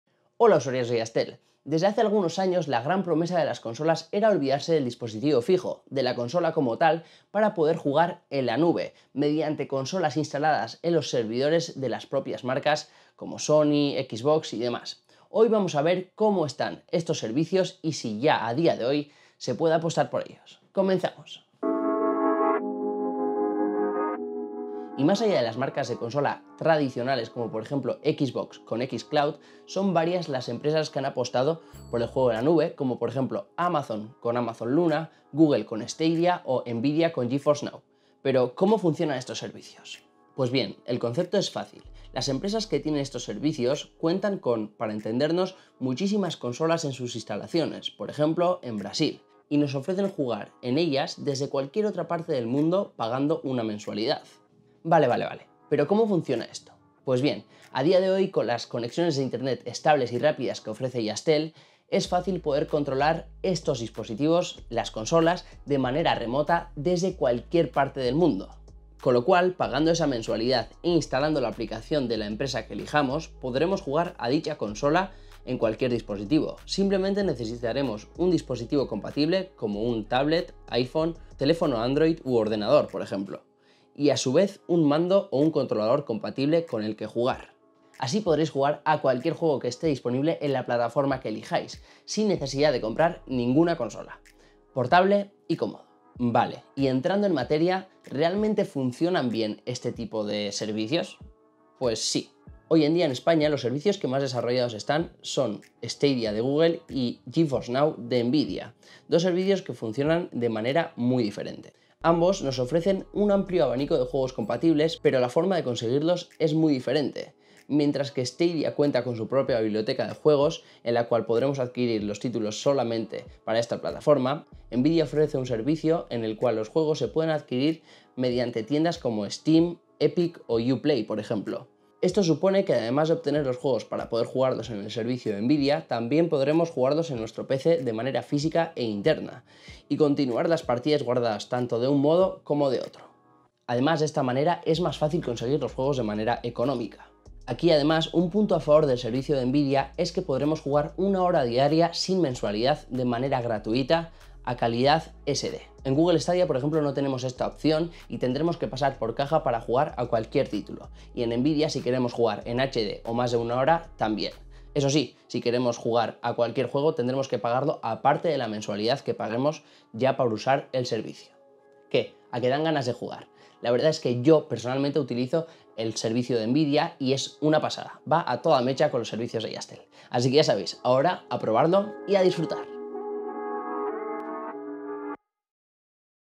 How to de Jazztel: Jugar en la nube ¿Cómo funciona?
¡Hola usuarios de Jazztel! Desde hace ya un tiempo, la gran promesa de las consolas era olvidarse del dispositivo fijo, de la consola física como tal, para jugar “en la nube” mediante consolas servidores de las propias empresas, ofreciendo siempre las mejores especificaciones pero sin cambiar nunca de consola. Hoy vamos a ver cómo funciona y qué opciones tenemos para jugar en la nube.